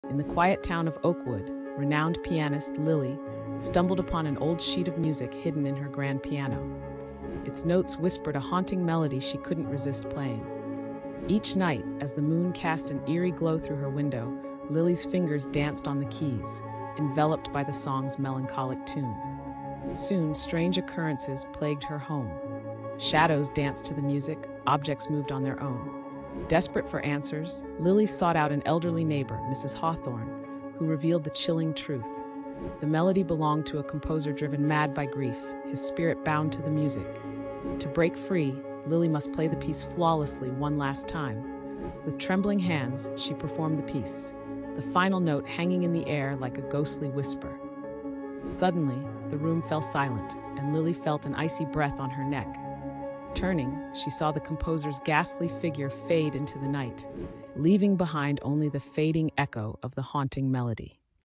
Uncovering a Musician's Eerie Dream
Short Stories
In the quiet town of Oakwood, renowned pianist, Lily, stumbled upon an old sheet of music hidden in her grand piano. Its notes whispered a haunting melody she couldn't resist playing. Each night, as the moon cast an eerie glow through her window, Lily's fingers danced on the keys, enveloped by the song's melancholic tune. Soon, strange occurrences plagued her home - shadows danced to the music, objects moved on their own. Desperate for answers, Lily sought out an elderly neighbor, Mrs. Hawthorne, who revealed the chilling truth. The melody belonged to a composer driven mad by grief, his spirit bound to the music. To break free, Lily must play the piece flawlessly one last time. With trembling hands, she performed the piece, the final note hanging in the air like a ghostly whisper. Suddenly, the room fell silent, and Lily felt an icy breath on her neck. Turning, she saw the composer's ghastly figure fade into the night, leaving behind only the fading echo of the haunting melody.